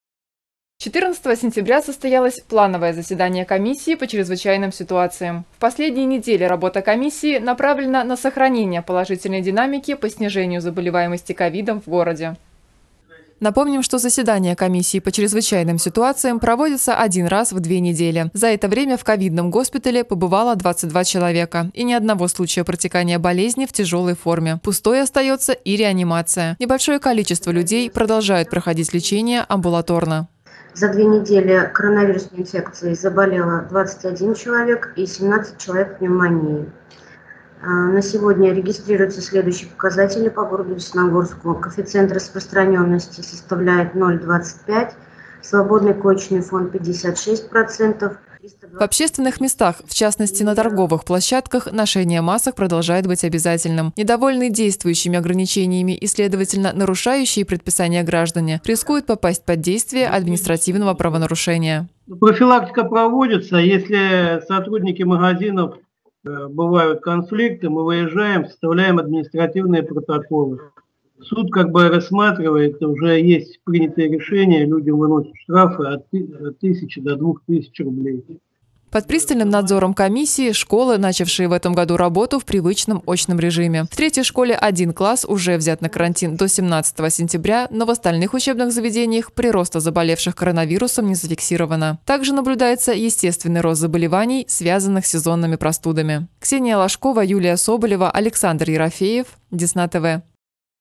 Десна-ТВ: Первое сентябрьское заседание КЧС
14 сентября состоялось плановое заседание комиссии по чрезвычайным ситуациям. В последние недели работа комиссии направлена на сохранение положительной динамики по снижению заболеваемости ковидом в городе.